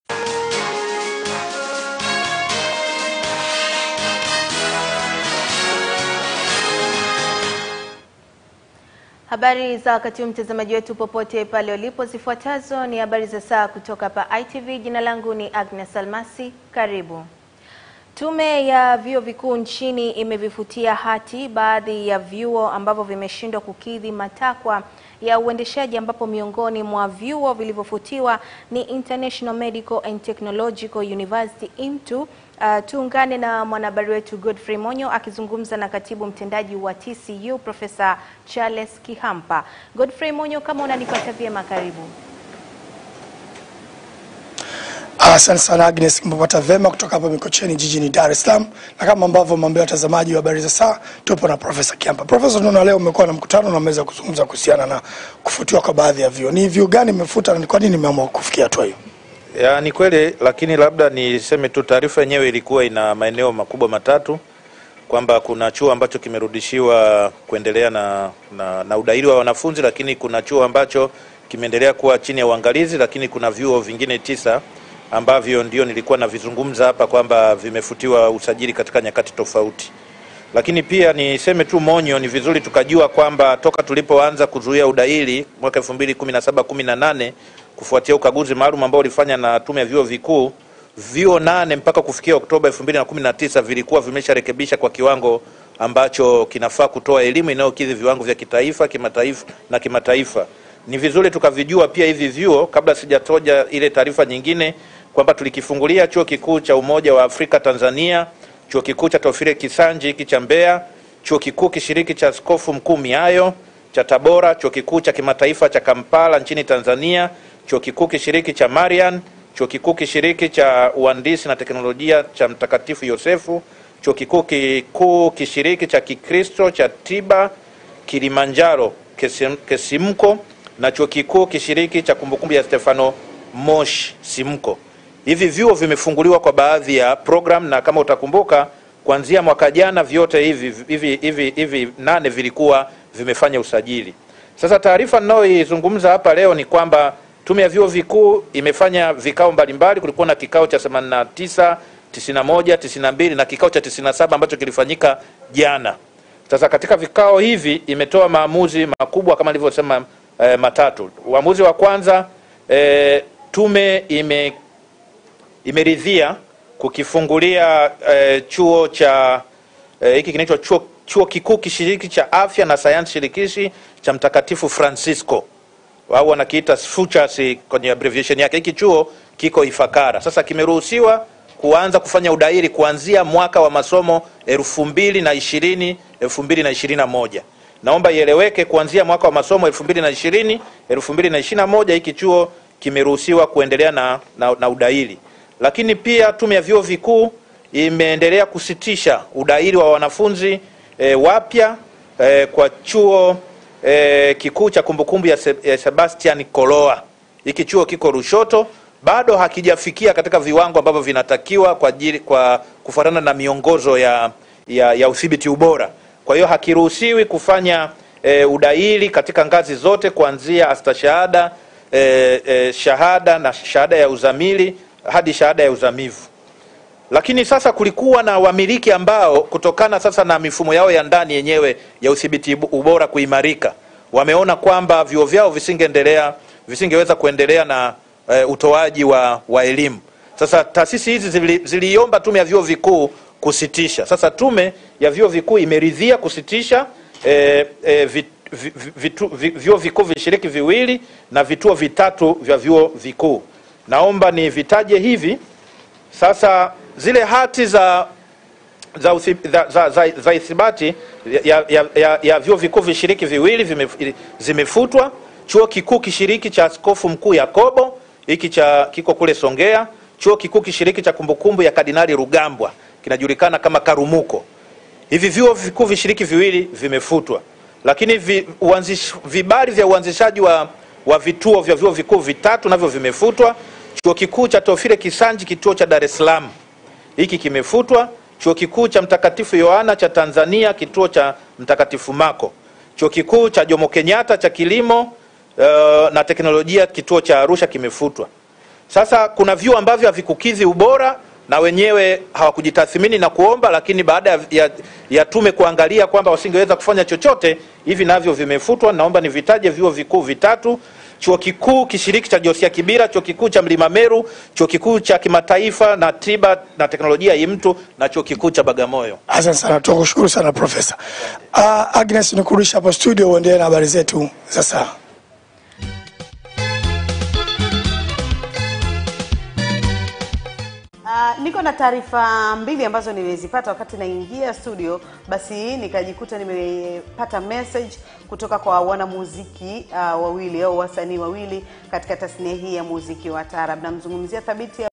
Vyuo vikuu vilivyofungiwa na kufutwa hivi hapa/vipo vilivyofunguliwa.
HABARI ZA SAA, SAA NNE NA DAKIKA 55...JANUARI 21,2020